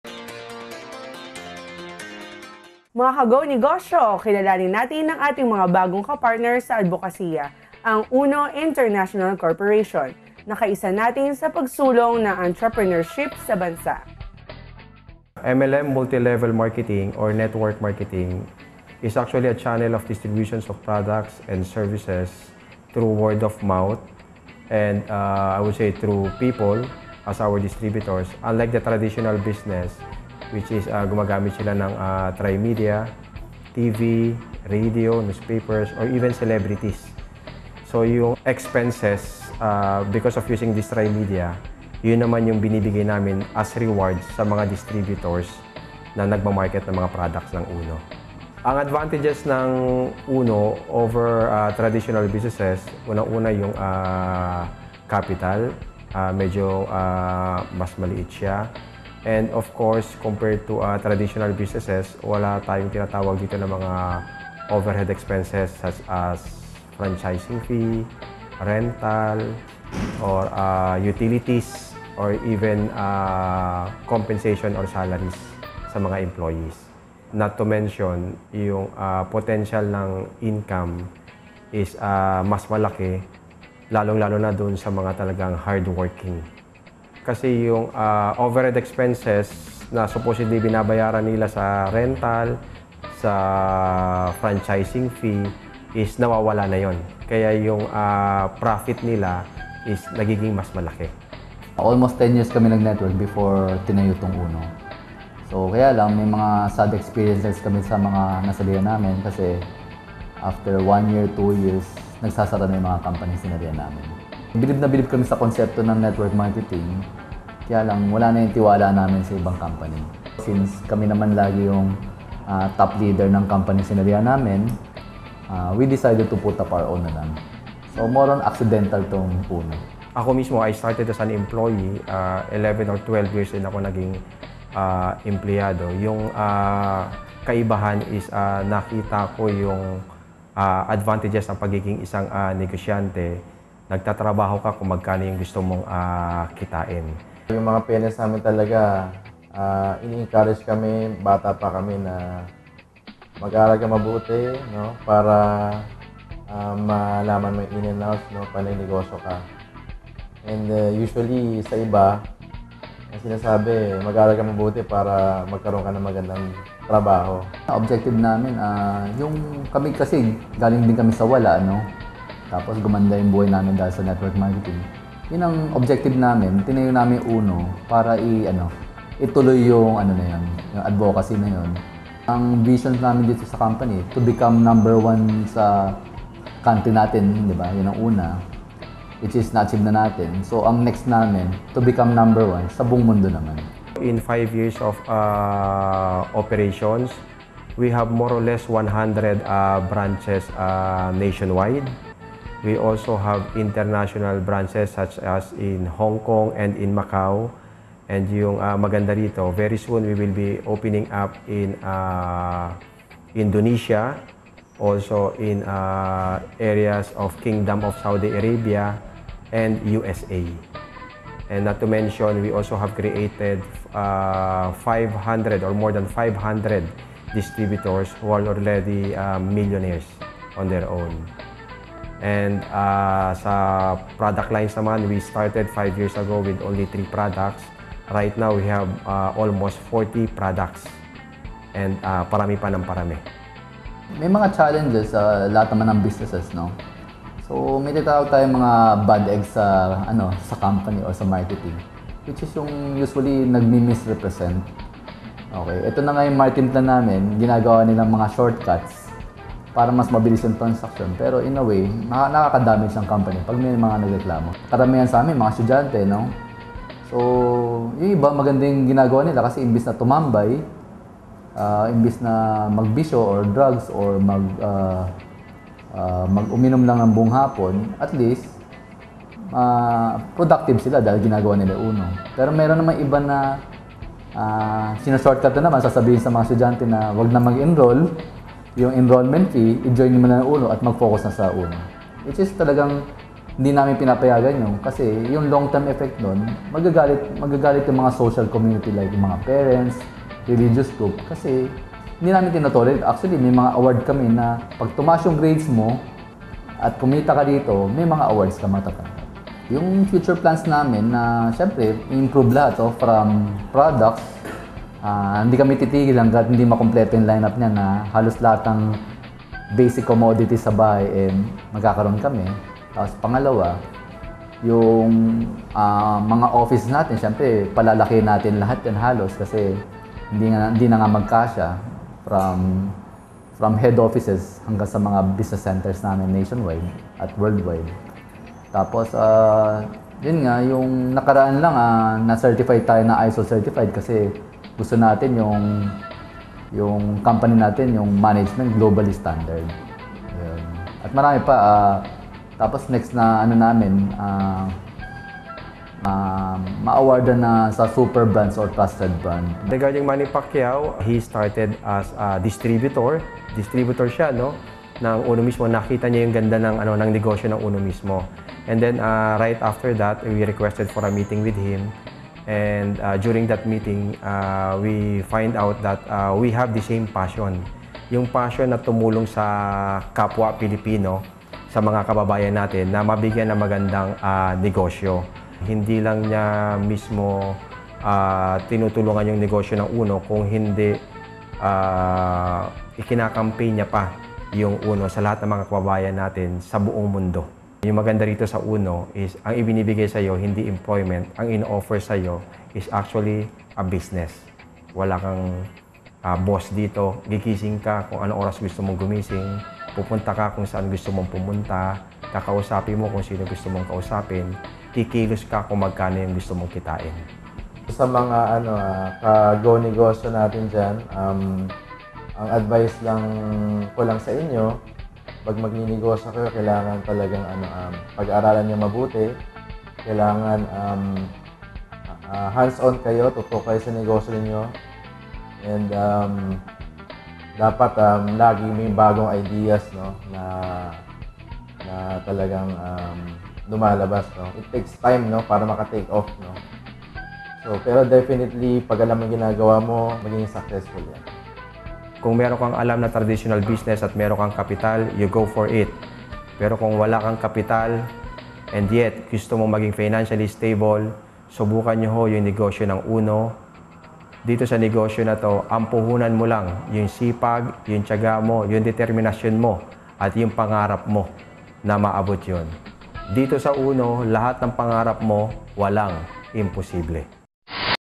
UNO Feat. GO NEGOSYO
Unlimited Network of Opportunities or UNO, The #1 Network Marketing Company in the Philippines Celebrated its 5th Year Grand Anniversary last June 9, 2012 at the Biggest and Grandest Concert venue, The Mall of Asia Arena. It was the BIGGEST EVENT with MORE THAN 20,000 +++ ATTENDEES FROM LUZON, VISAYAS, MINDANAO, HONG KONG, JAKARTA AND THE USA!